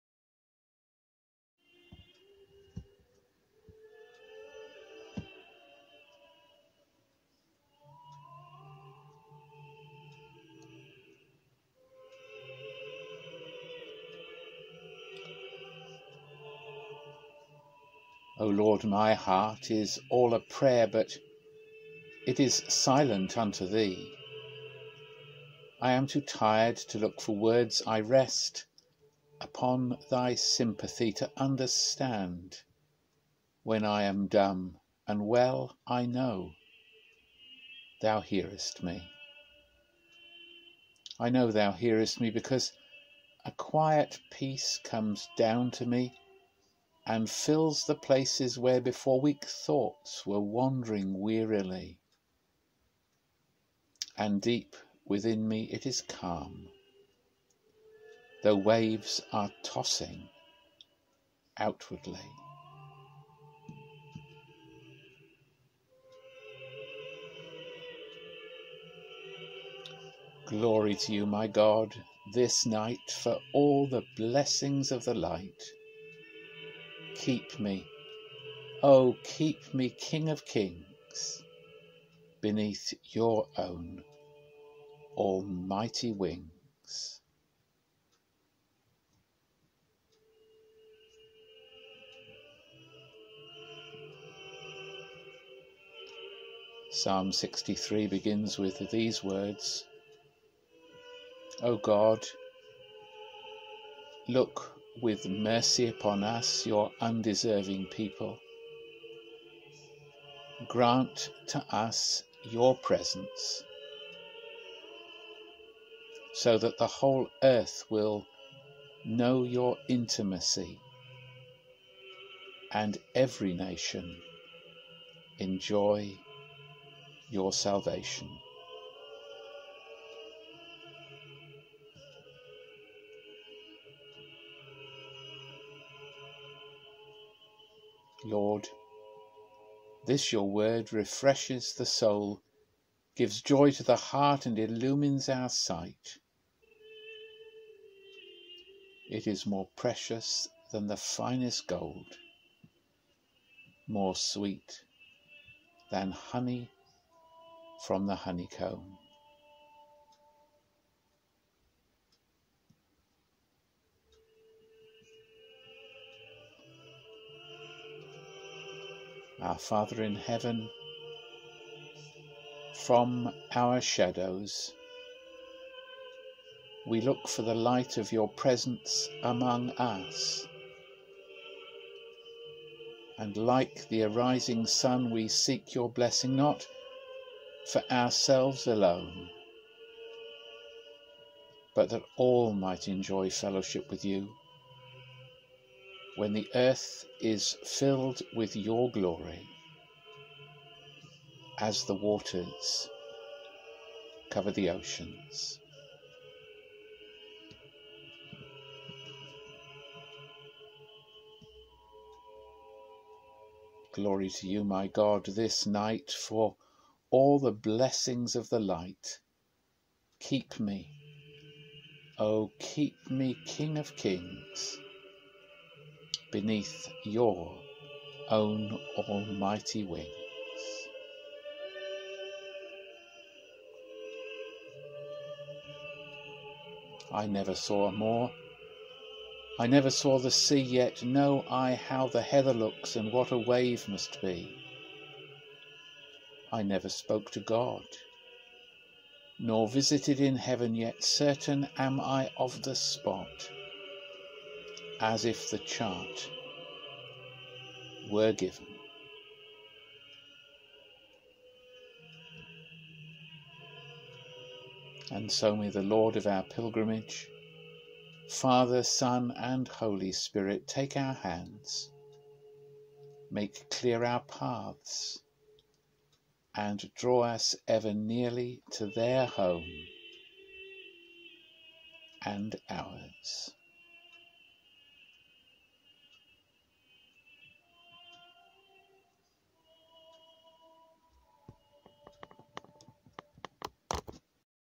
Night Prayer 3rd October 2020.
A short devotion led by Stephen Dray.